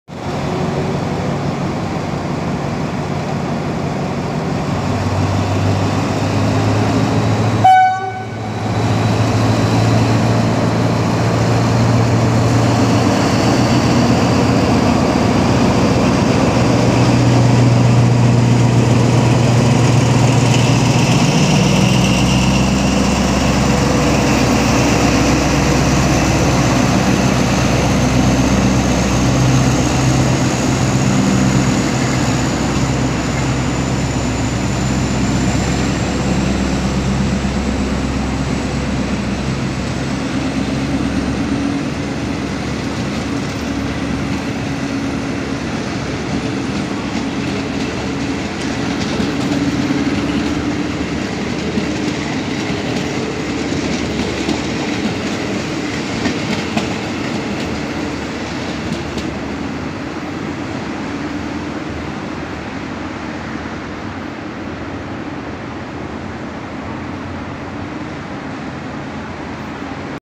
ขบวน 301 ALD.4303 ออก กม.19
ALD.4303 ทำขบวน 301 ถึงป้ายหยุดรถการเคหะ กม.19 เวลา 17.19 น. ออก 17.20 น. ตามเวลา
9 ม.ค.65